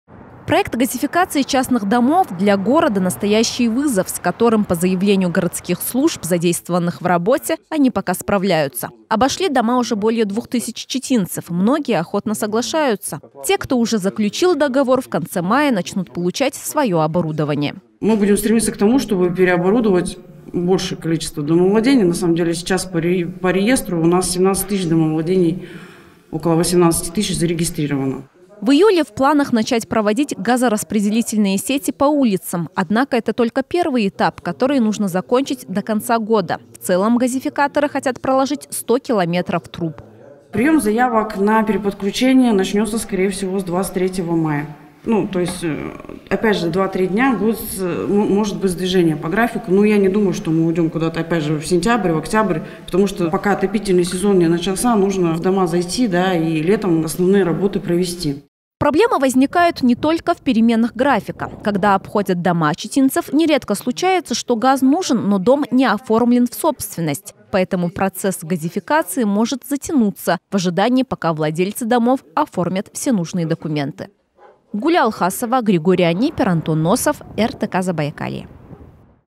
В конце мая в частных домах Читы начнут устанавливать газовое оборудование
Стали известны новые подробности газификации Читы, которая идёт в  городской черте полным ходом. Более 2000 домовладений  специалисты уже обошли, а в конце мая всем, кто согласился провести газ, начнут устанавливать оборудование.